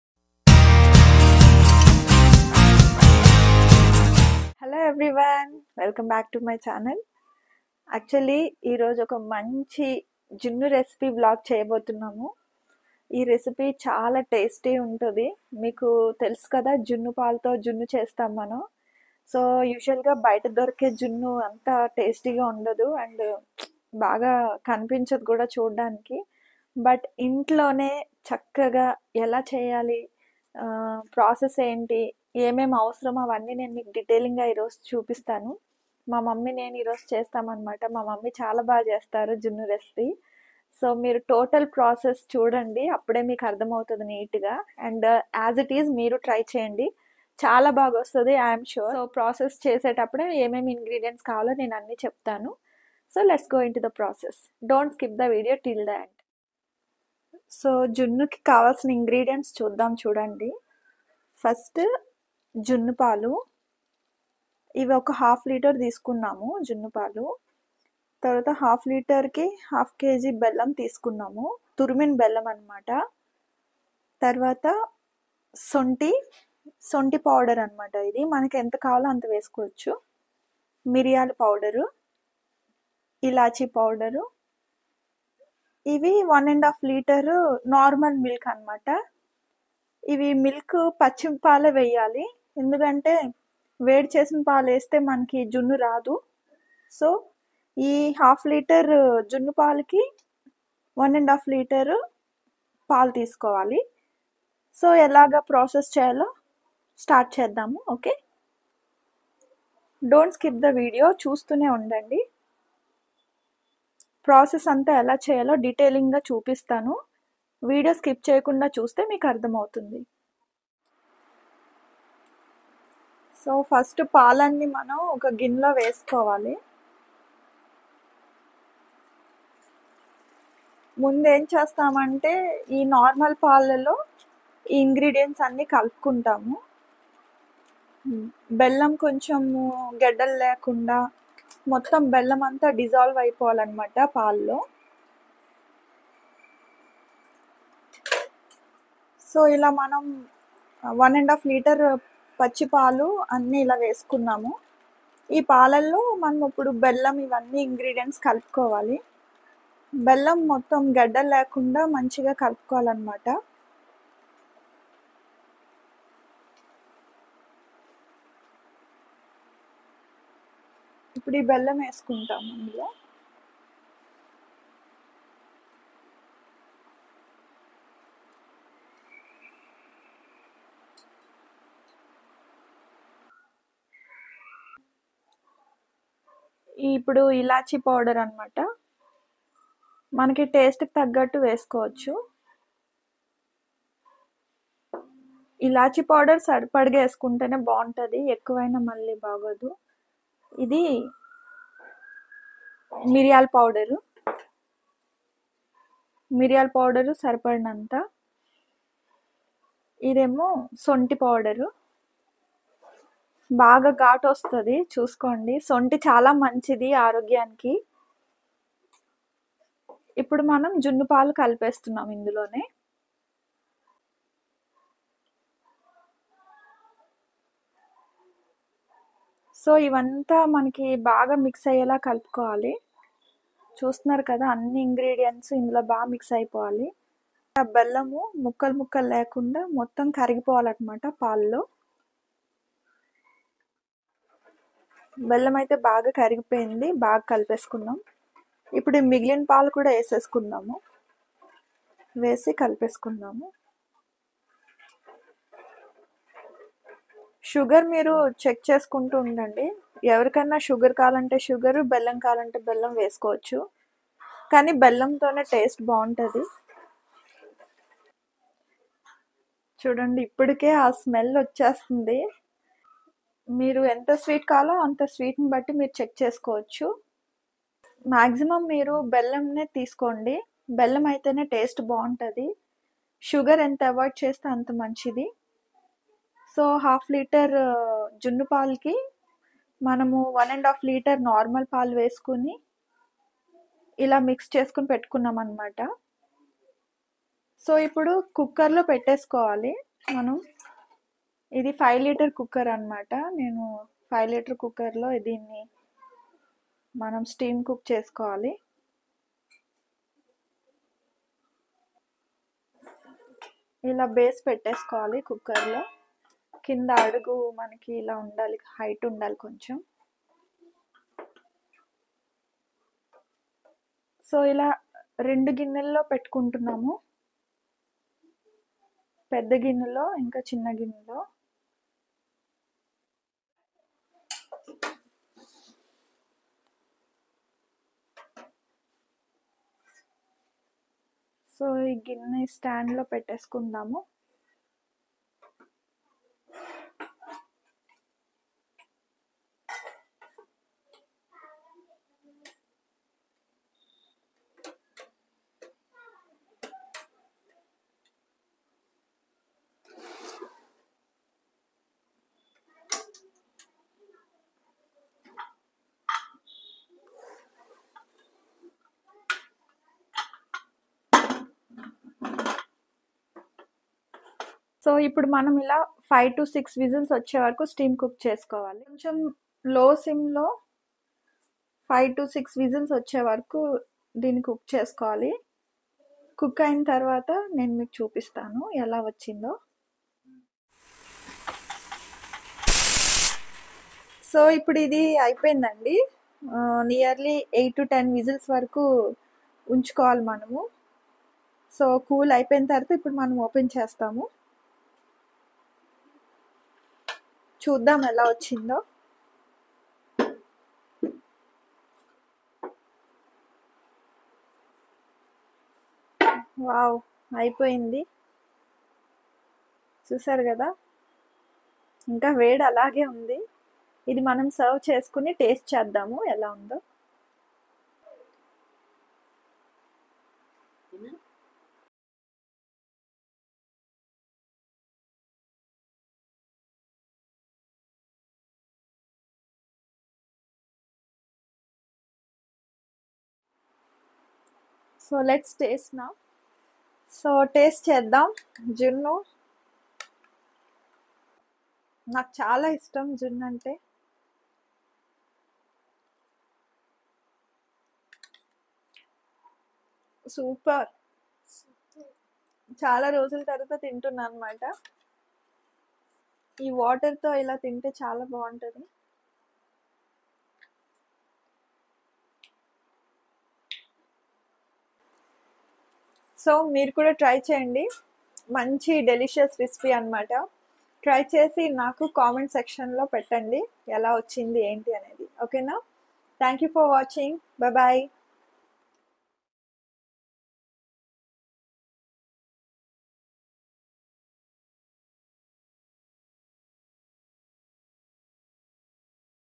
Delicious Junnu Recipe l అమ్మా జున్ను చేస్తున్నారు l మీరు try చేయండి 👍 taste మీరు ఎప్పటికీ వదలరు 😋👍
Here is a video on Junnu recipe made with Colostrum Milk.
Ingredients:
1.Colostrum milk (1/2 litre)
2.Normal milk (1.5 litres )
3.Jaggery (1/2 kg)
4.Dry ginger powder
5.Pepper powder
6.Cardamom powder